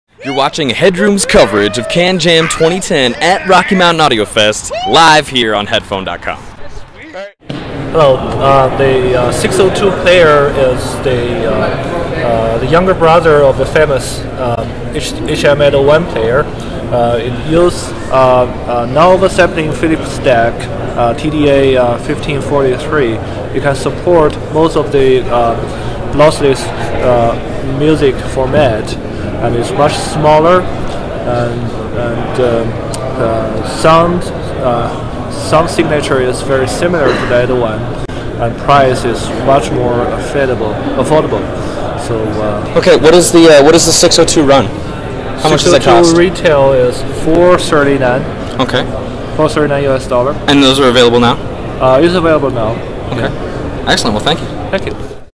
RMAF 2010: HifiMan HM-602
Fang from HiFiMan announced his new HM-602 player at RMAF 2010 this year. This year's CanJam had some great releases, and this high quality audio player with a similar sound signature to it's older brother was one of the big ones. Don't forget that we've got a ton of coverage this year, and you can check it all out here on youtube